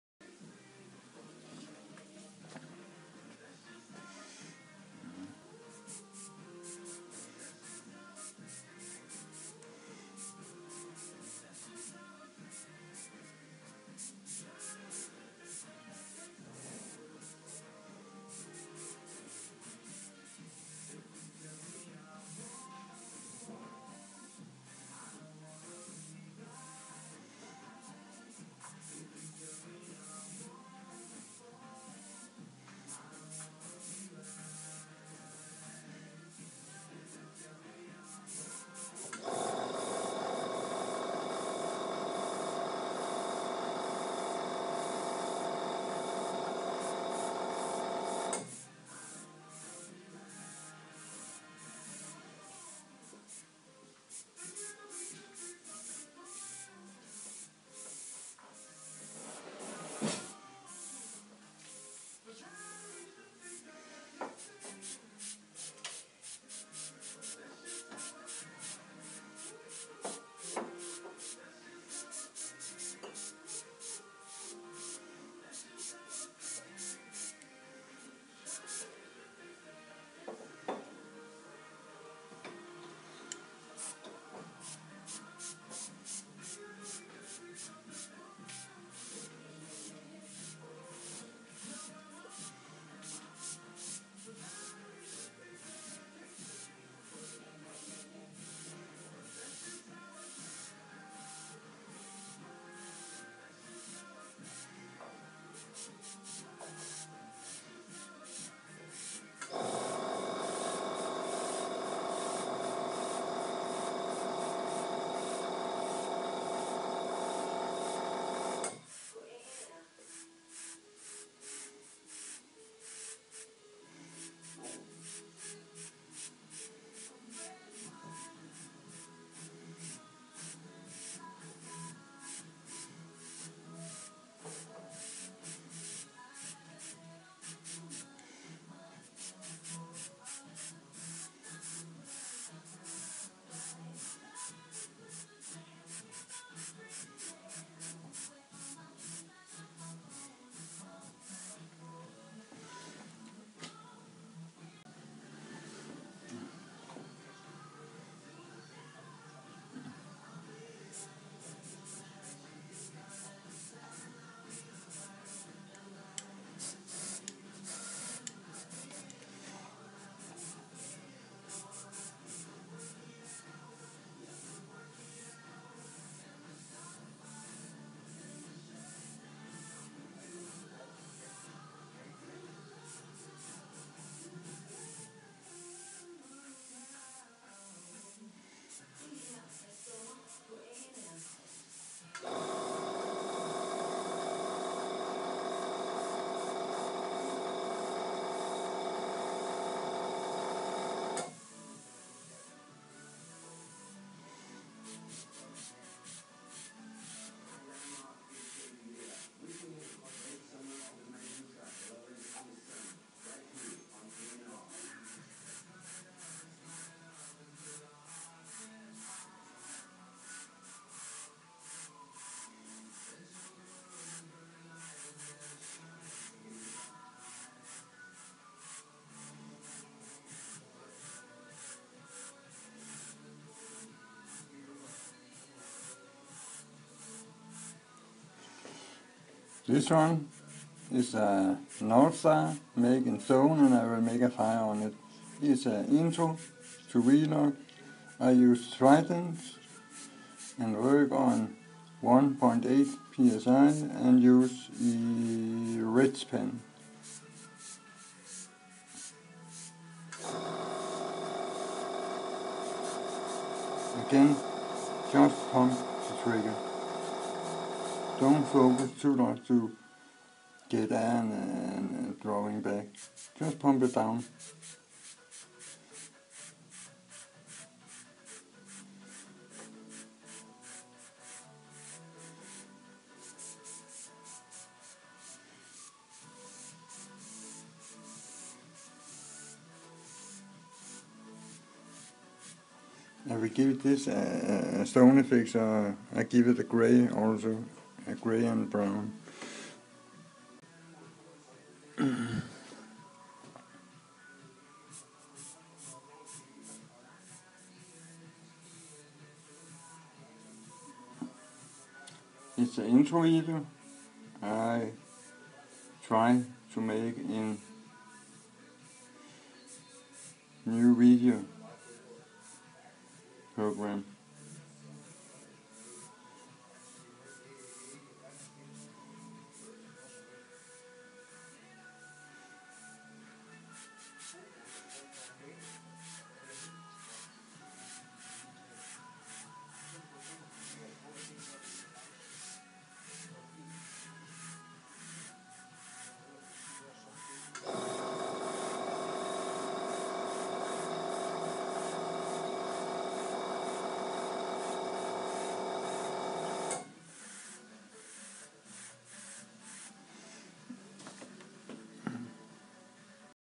Airbrush Star in Stone
How to Stone Star in fire learn and see How to do details,shading and highlight.
By Airbrush-dk.dk